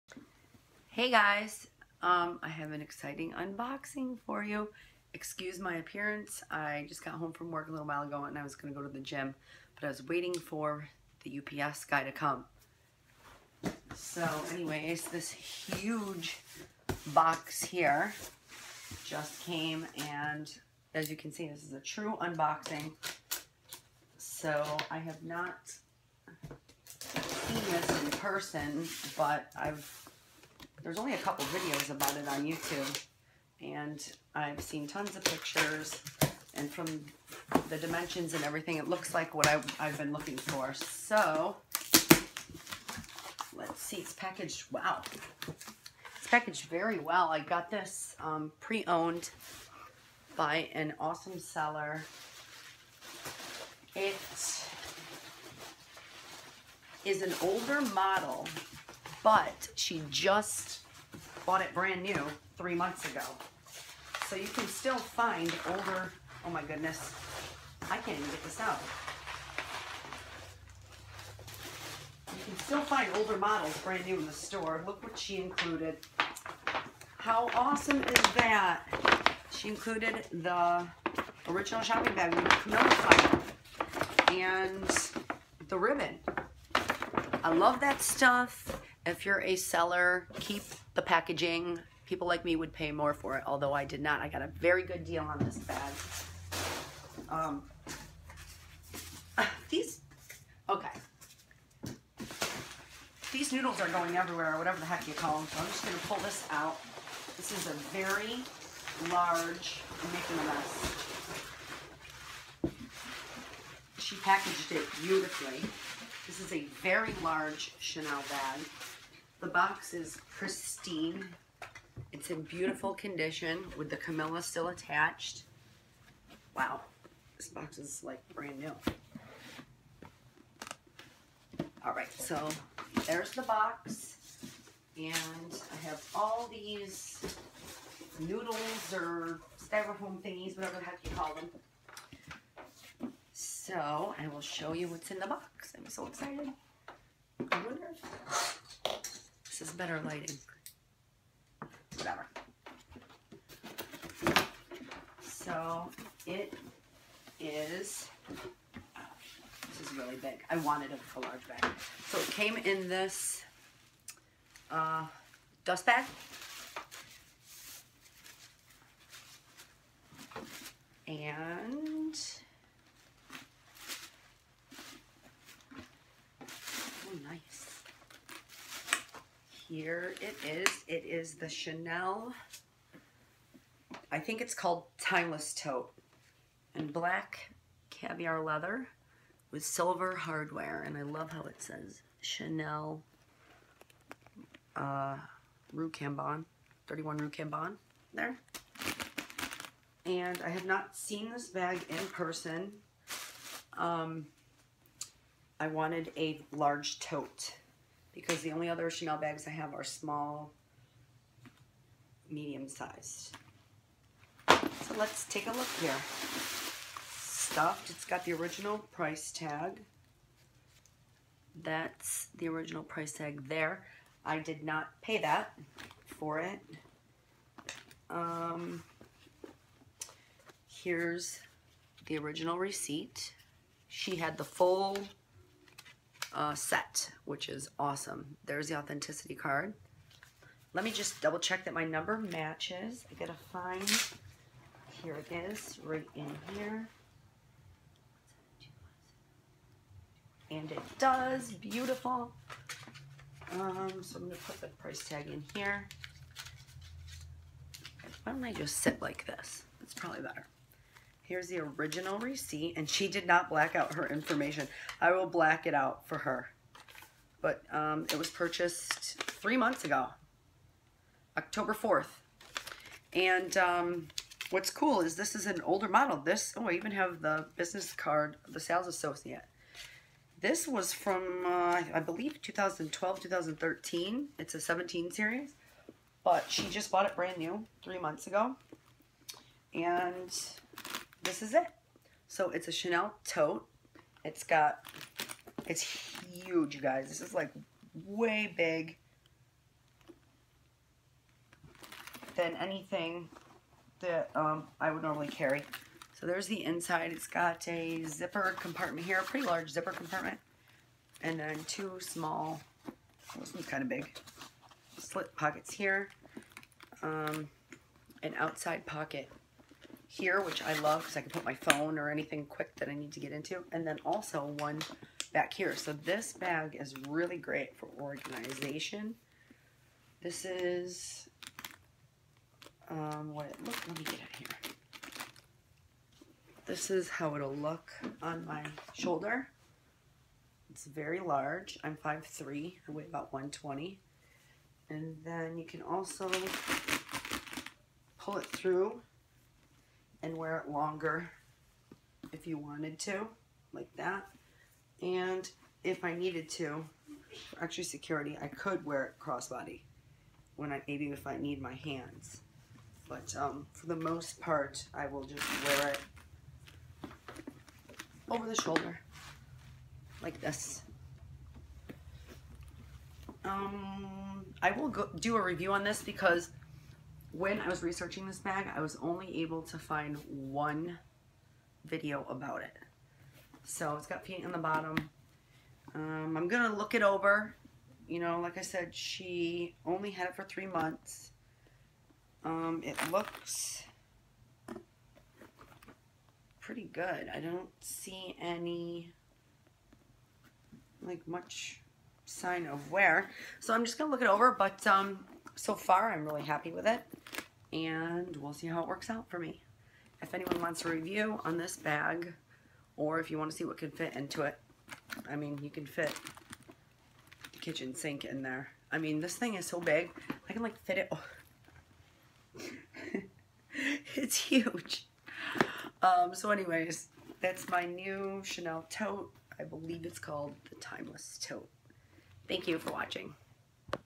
Unboxing and reveal of my new Chanel Timeless Tote bag!
Hey guys! This is my unboxing of my latest Chanel purchase. It is a 17 series from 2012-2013 I believe, however it was purchased brand new from the store a few months ago.